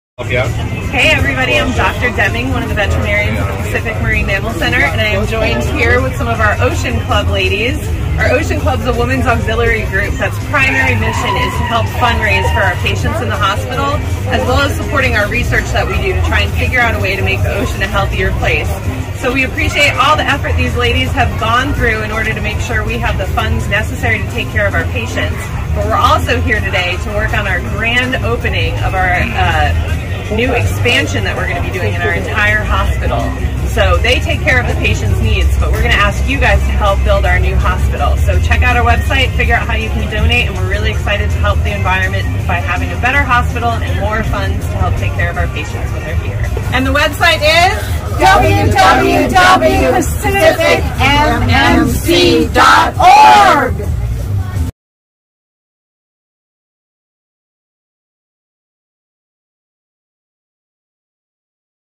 PEOPLE at PMMC | Ocean Club
AUDREY4CARE: Caring for Animals, Respecting the Earth," began with a personal letter of encouragement from President Barack Obama, inspiring efforts to raise awareness and promote environmental care. AUDREY4CARE is honored to have been recognized as the National Winner of the President's Environmental Youth Award and awarded the Junior Philanthropist of the Year by the Pacific Marine Mammal Center.